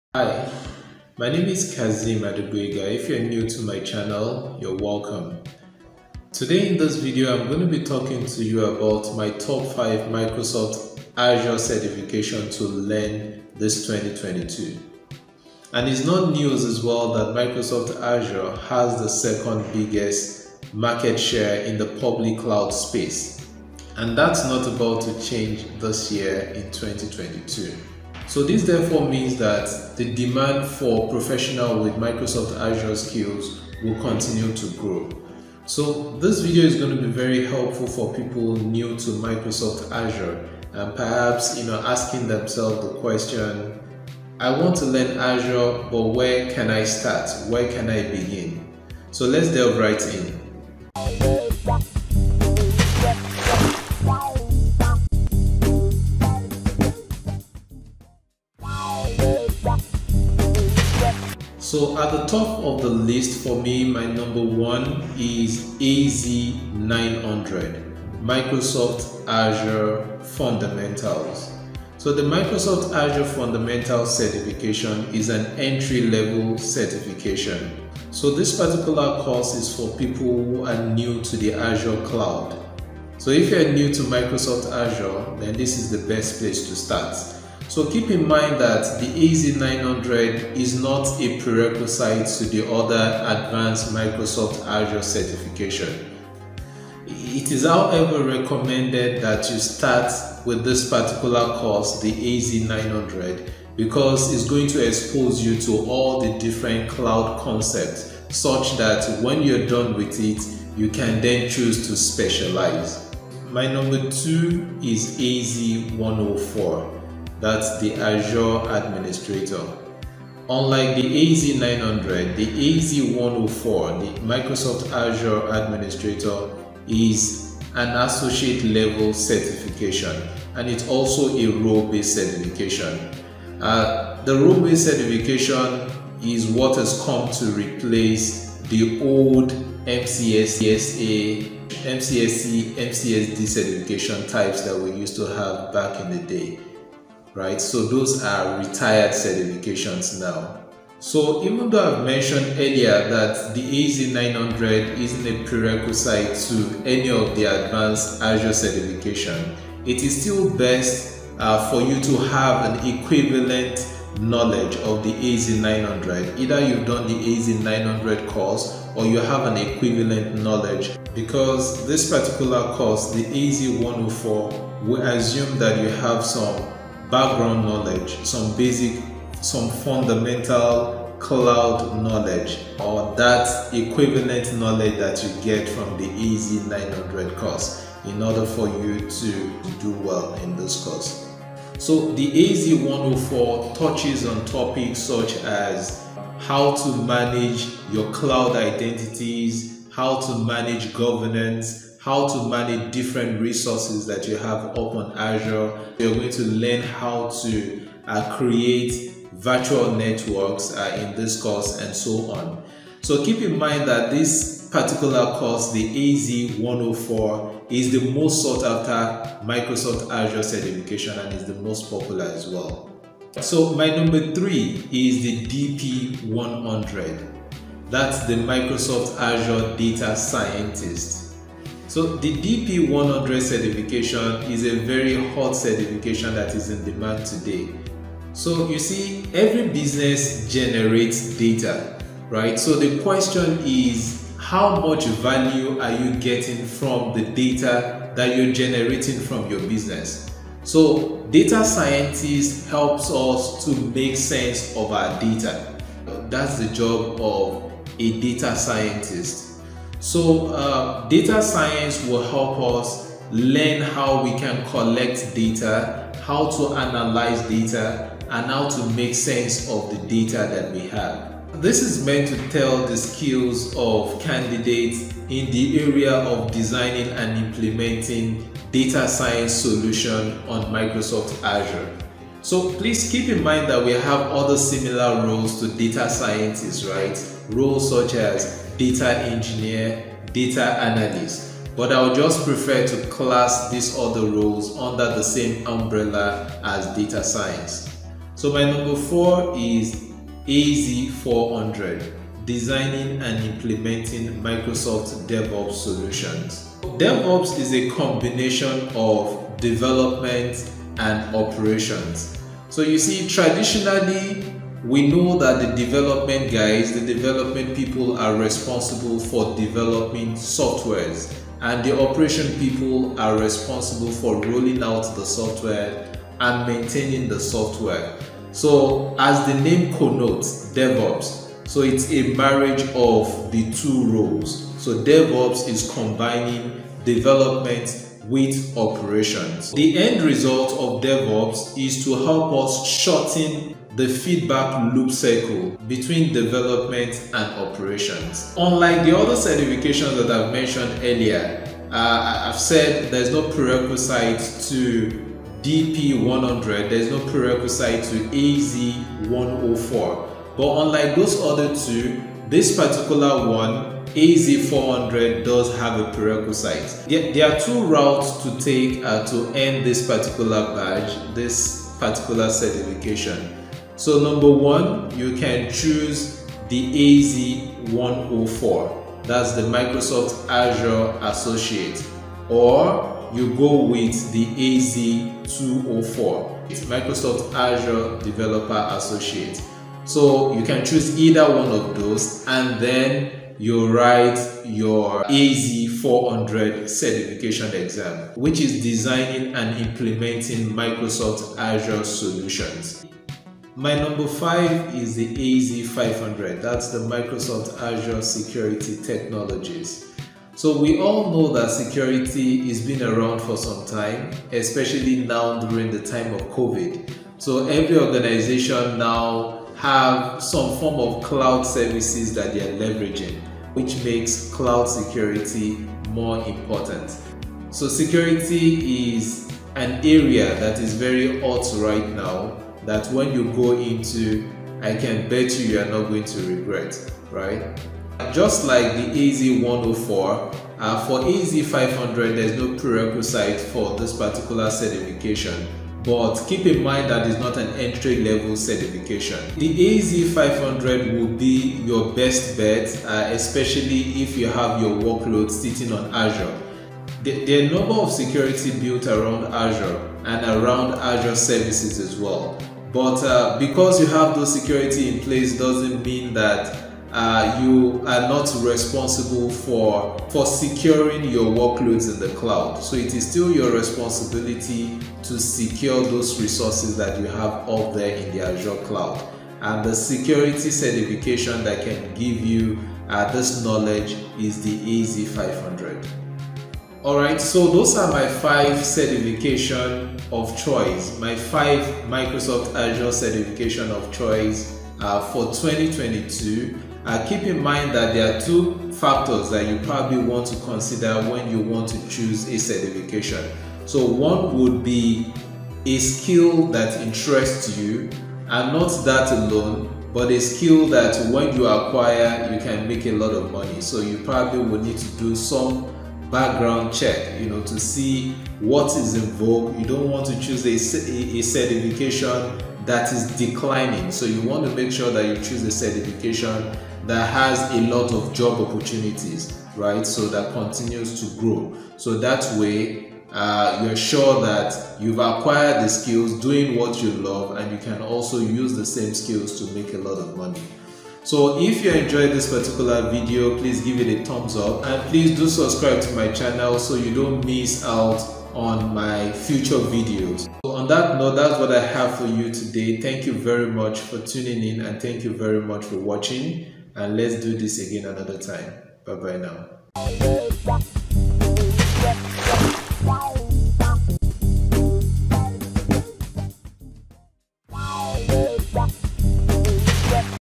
The Top 5 Azure Certification for 2022 (for Beginners)
Microsoft Azure skills are becoming more valuable in today's competitive market. Microsoft Azure is a renowned cloud computing platform, and it has aided businesses in streamlining their IT infrastructure.

In this episode, we look at 5 important skill sets and guidelines to keep in mind when considering a role in Cloud Computing.

Below are the skills and Certifications covered:

1. AZ-900 Azure Fundamentals
2. Az-104 Azure Administrator Associate
3. DP-100 Azure Data Scientist
4. AZ 400 Designing & Implementing Microsoft DevOps Solutions
5. AZ 500 Azure Security Technologies

⏱ Timestamps ⏱
0:00 Introduction
1:02 AZ-900
1:53 AZ-104
3:42 DP-100
4:57 AZ-400
6:43 AZ-500
9:19 Outro